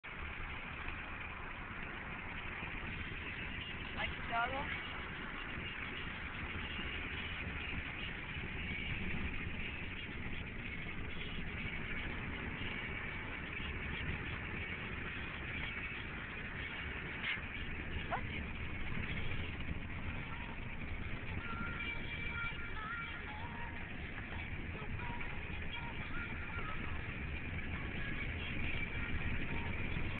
Siberian Husky Nakita Phanco wearing her new k-9 optics
Anytime we go cruzin in the car we make Nakita wear her shades.  She used to wear doggles and those wore out.  Now she is sportin her new K-9 optics on the way to the doggie park.  She loves wearing them.